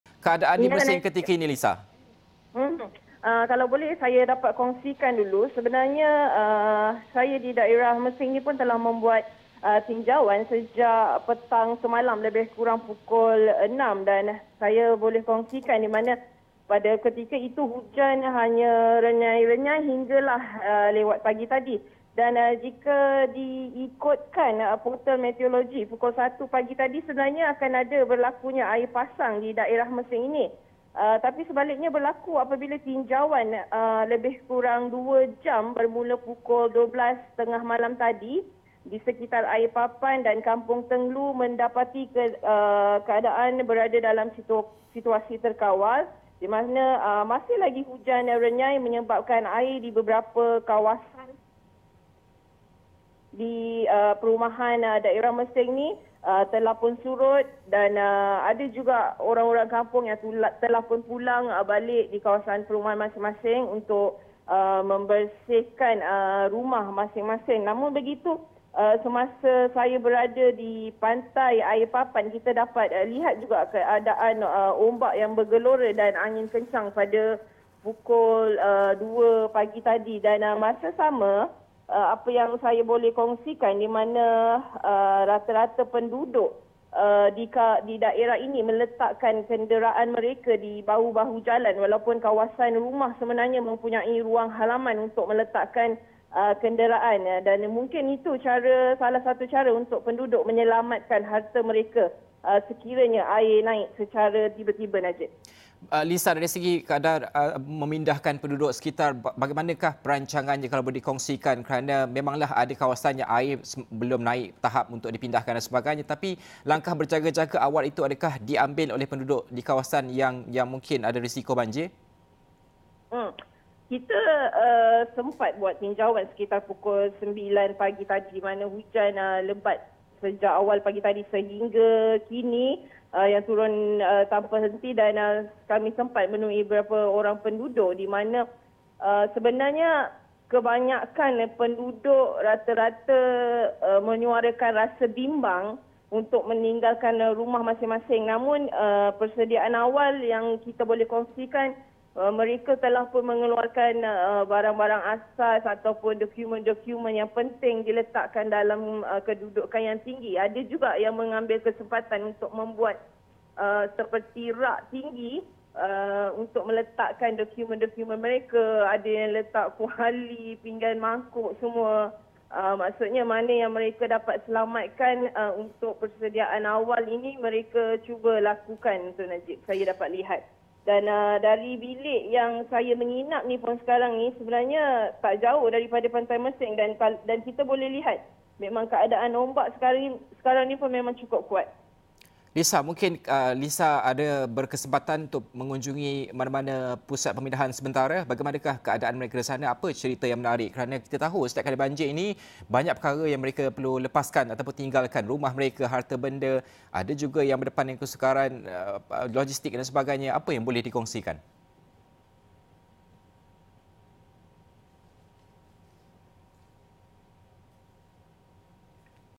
Situasi banjir di Mersing
Di talian Wakil Astro AWANI Johor, Ernalisa Othman.